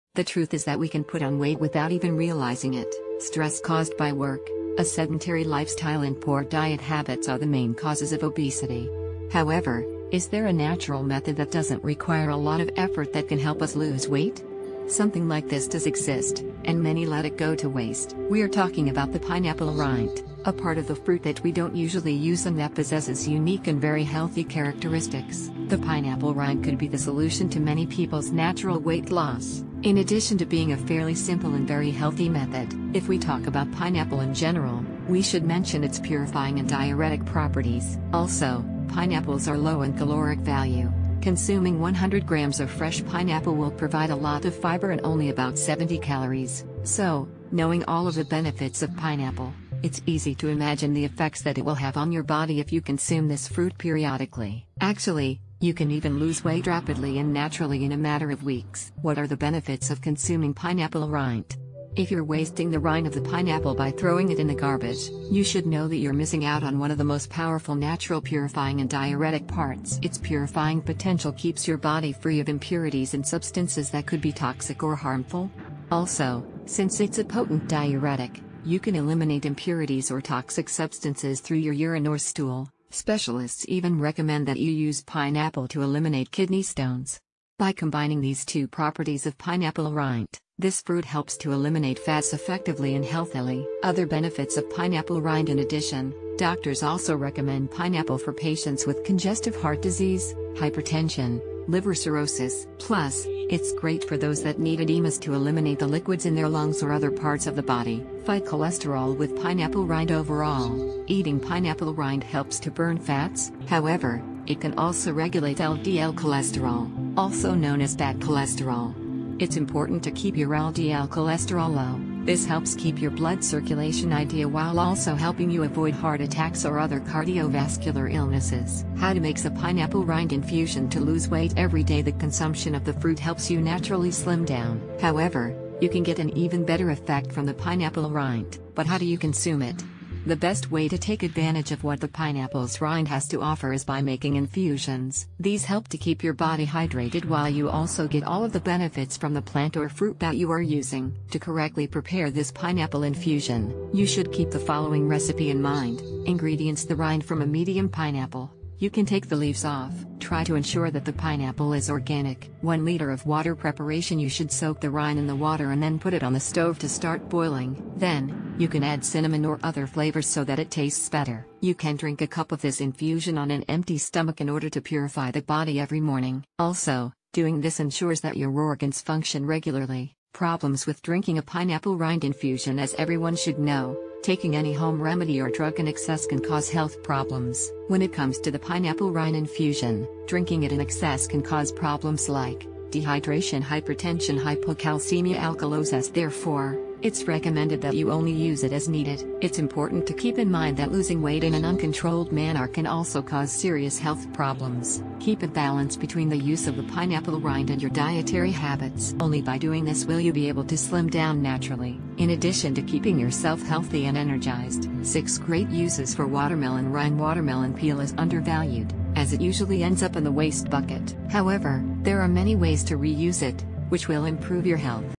Lose Weight Every Day with Pineapple Rind
The truth is that we can put on weight without even realizing it. Stress caused by work, a sedentary lifestyle and poor diet habits are the main causes of obesity.  However, is there a natural method ...